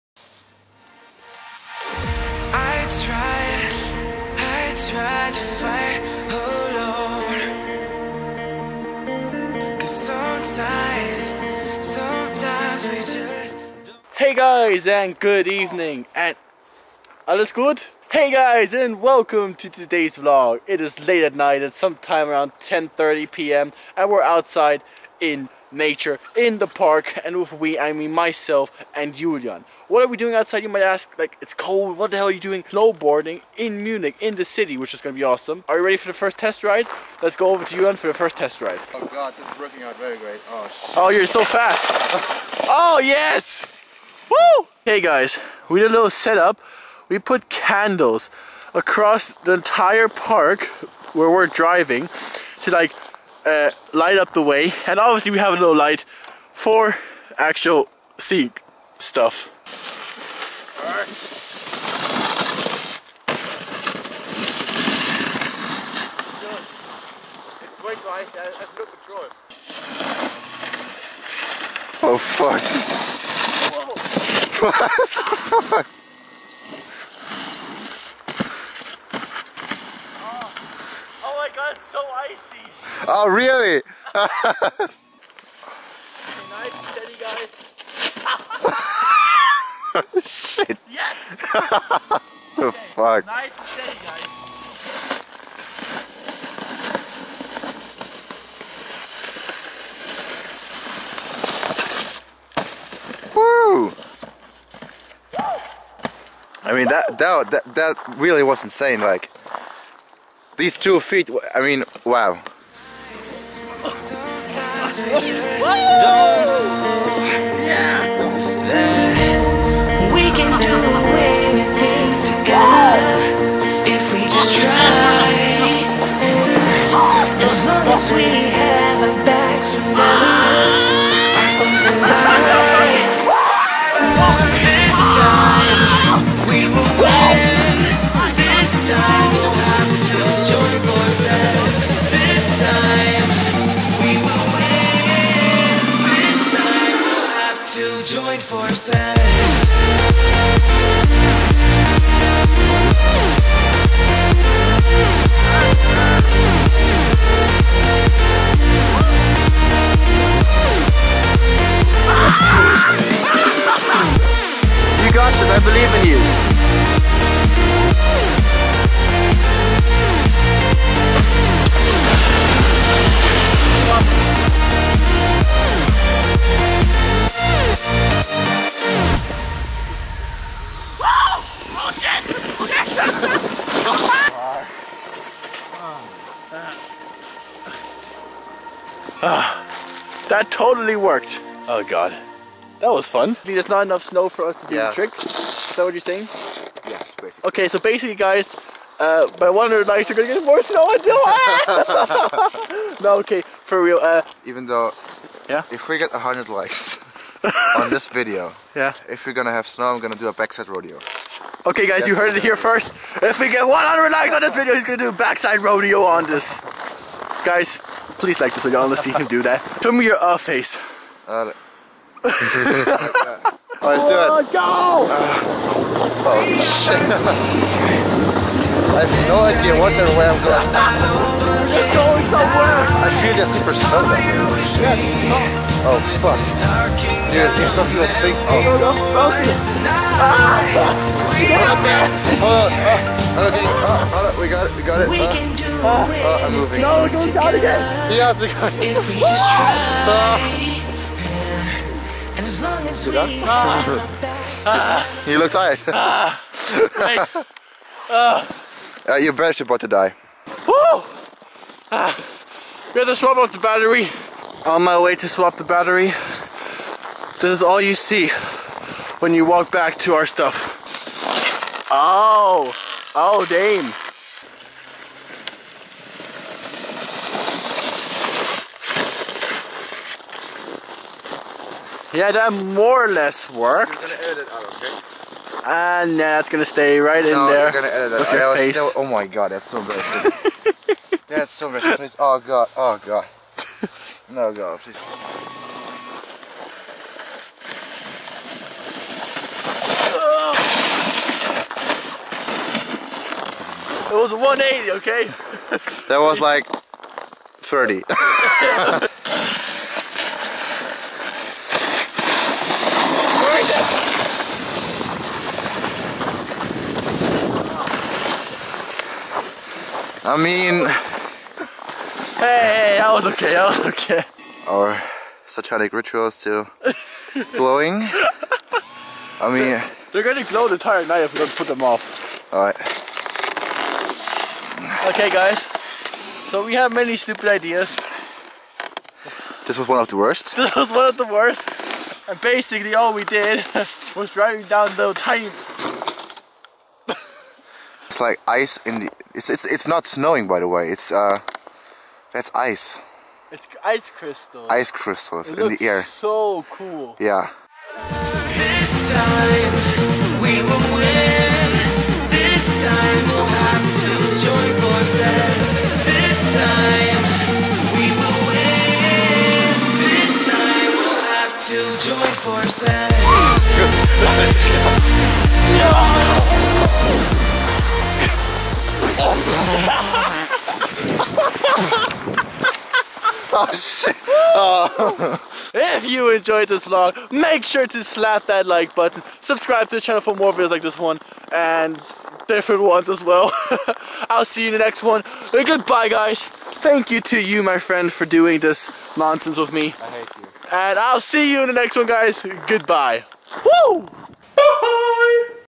Snowboarding in Munich at Midnight!
Hello, my name is Alex and im just a "normal" Guy doing some vlogs for you guys!
Today i was out W/ Julian and we Went snowboarding in munich!

➔ Day 6 of the Vlog